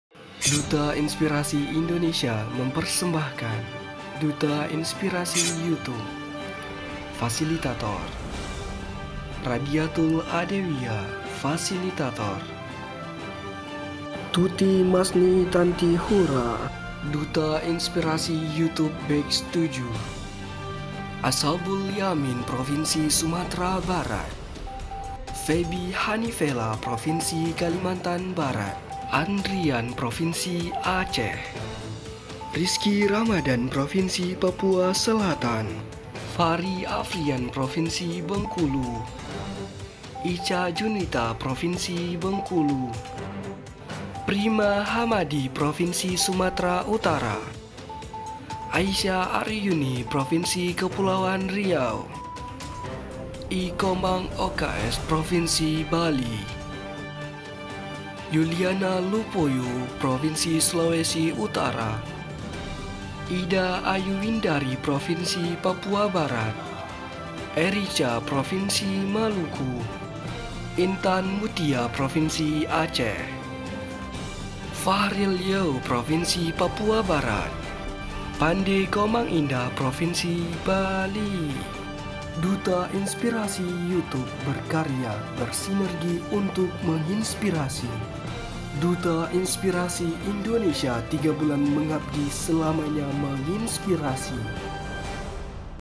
DUTA INSPIRASI INDONESIA YOUTUBE BATCH 7‼️ | (Profil Perkenalan)
Ini saatnya kami menunjukan, bahwa kami ada untuk melanjutkan perjuangan, menguatkan pergerakan, membawa perubahan.

Orang-orang hebat saling berkolaborasi memberikan pengabdian dan pengembangan demi terciptanya kebermanfaatan. Tentu dibalik hal itu, terdapat mereka yang berada dibalik layar. Penasaran siapa mereka, para figur sigap dibalik layar?

Maka dari itu, kami persembahkan "Video Profil Duta Inspirasi Indonesia Youtube Batch 7"

Selamat menyaksikan